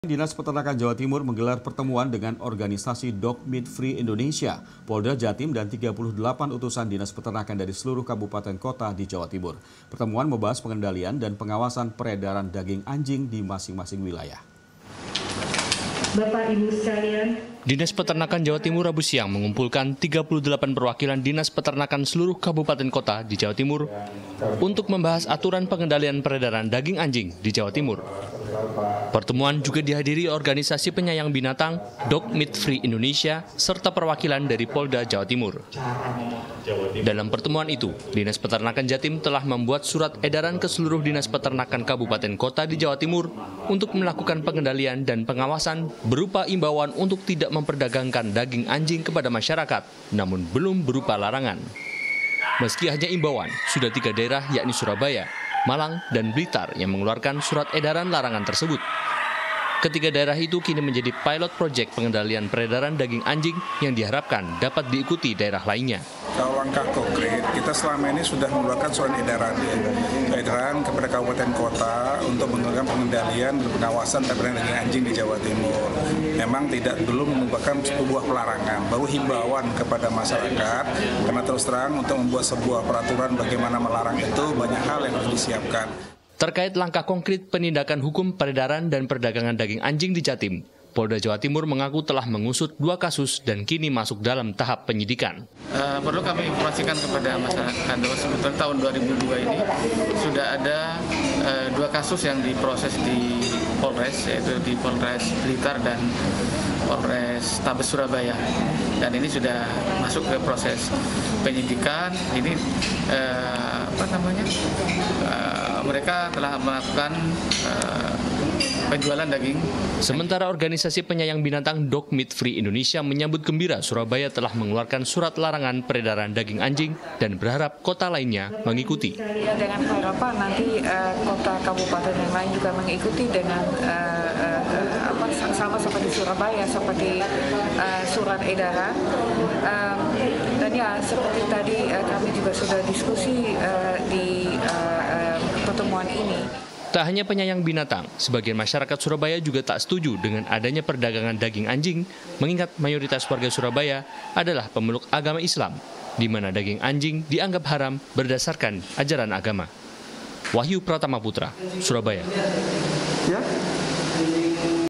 Peredaran Daging Anjing
Dinas Peternakan Jawa Timur Menggelar Pertemuan Dengan Organisasi Dog Meat Free Indonesia, Polda Jatim Dan 38 Utusan Dinas Peternakan Dari Seluruh Kabupaten Kota Di Jawa Timur. Pertemuan Membahas  Pengendalian Dan Pengawasan Peredaran Daging Anjing Di Masing-Masing Wilayah.